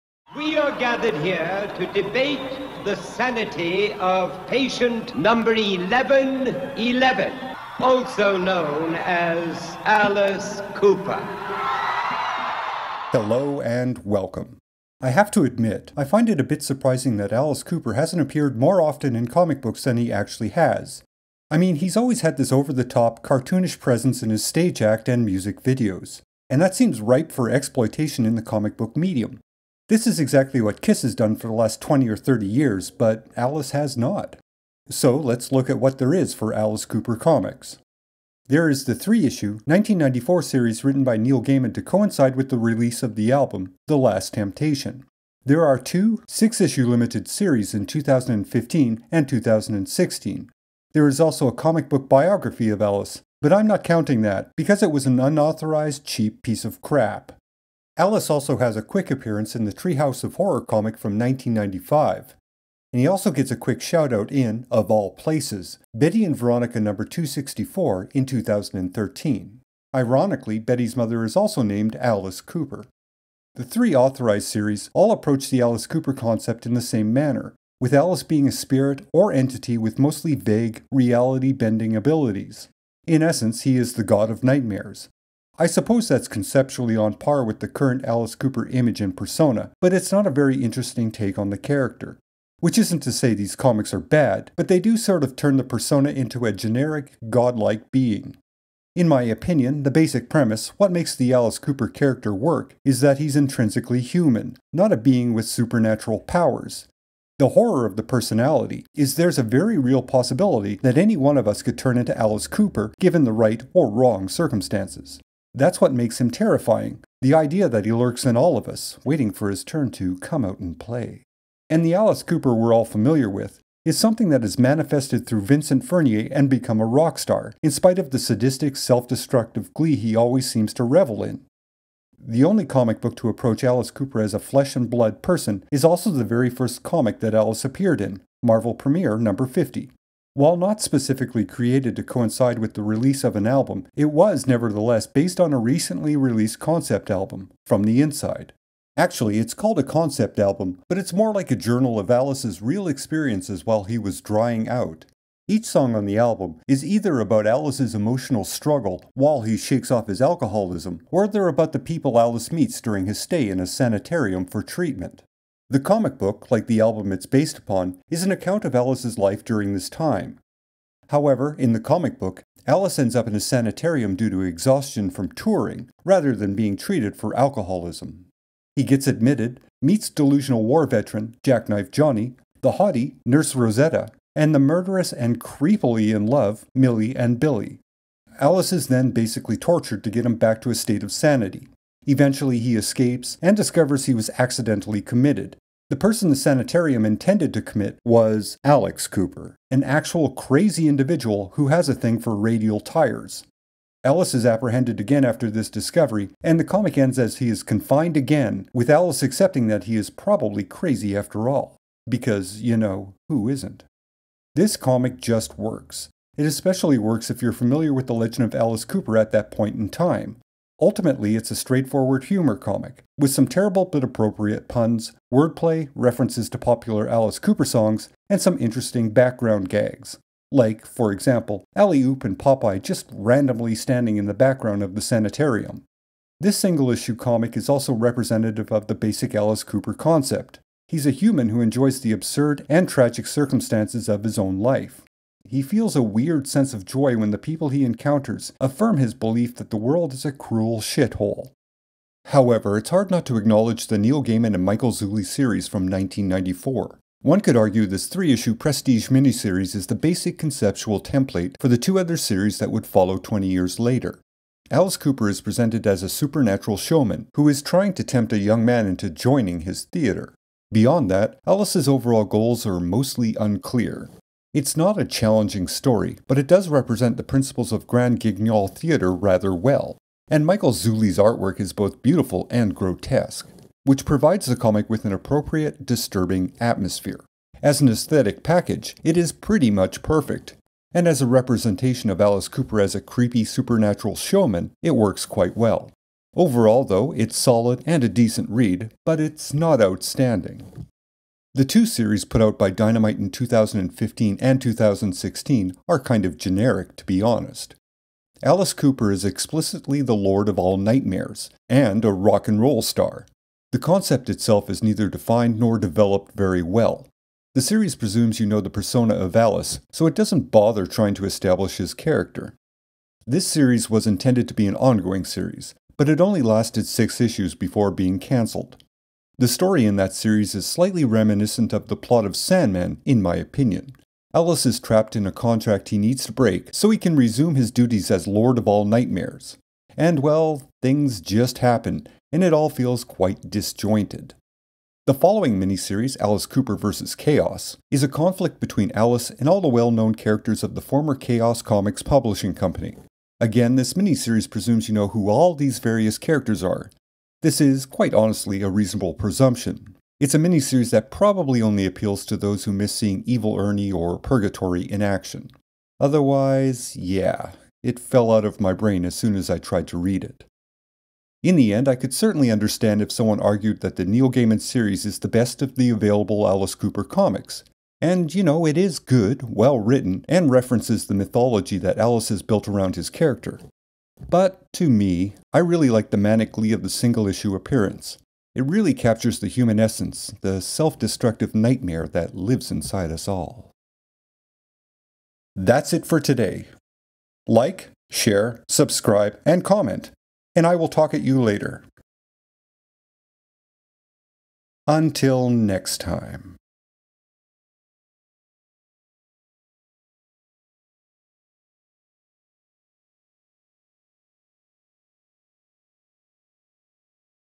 Alice Cooper In Comic Books
A look at all the comic book appearances by the rock entertainment legend, Alice Cooper.

(Get early, ad free access to videos.)